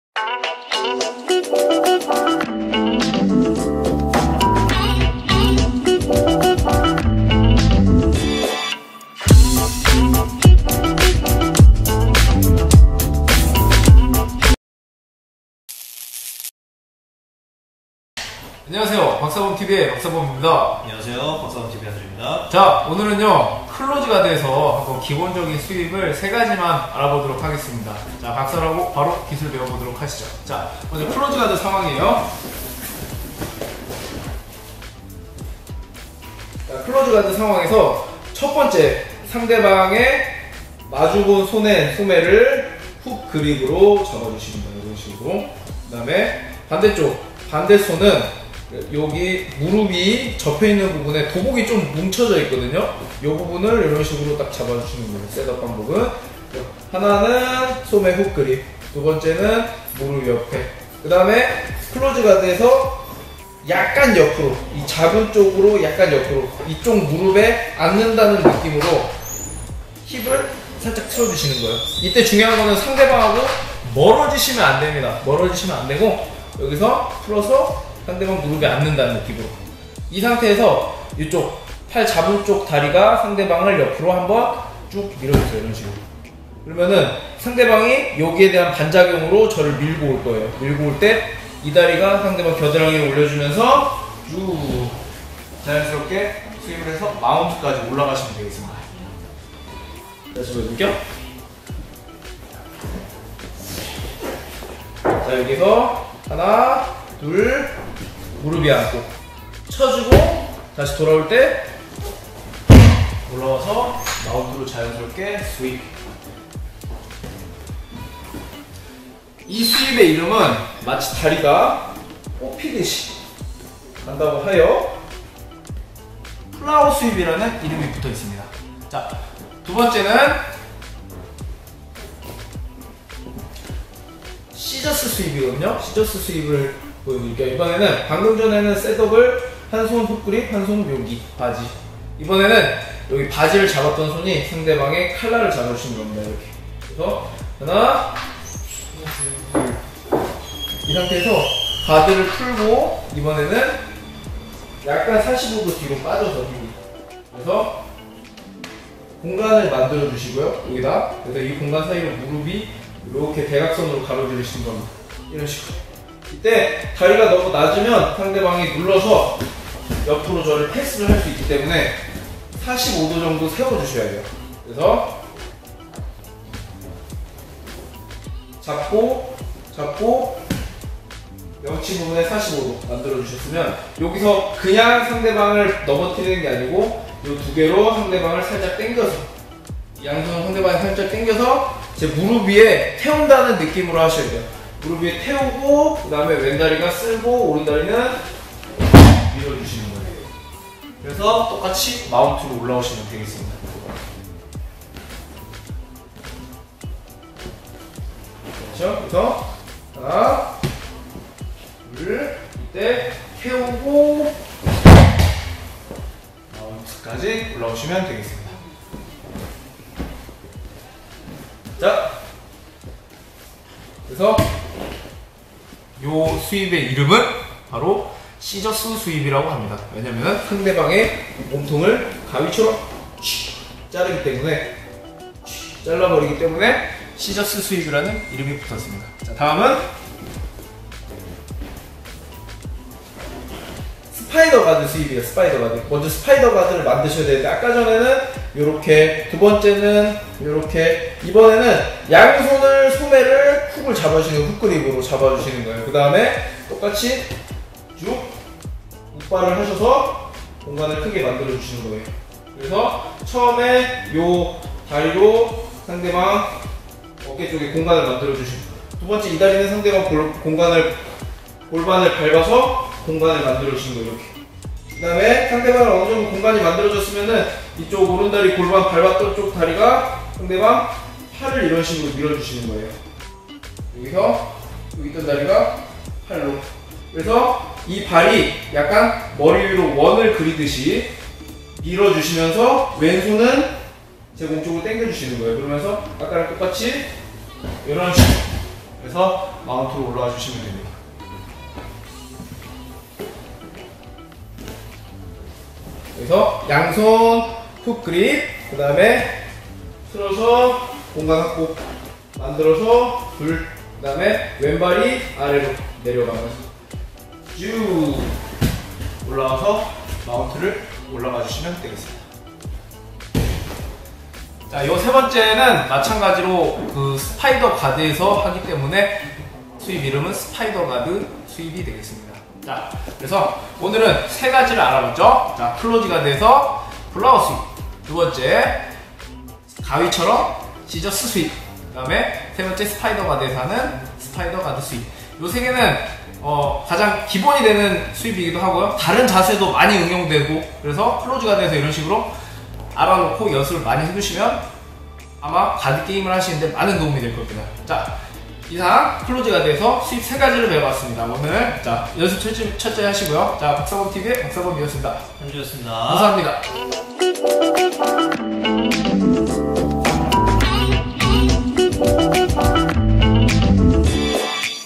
[박사범 주짓수 교실]화려한거 추구하지마라! 독이다! 클로즈가드에서 시작하는 확실한스윕 3가지(closed guard confident sweep)
너무 화려한것보다는 가끔은 기초에 충실한 공격이 먹힐때가 있어요.

주짓수, 호신술, 스윕, 클로즈가드, 스파이더가드, 시져스스윕, 플라워스윕, bjj, jiujitsu, selfdefense, sweep, closedguard, spiderguard, scissorssweep, flowersweep,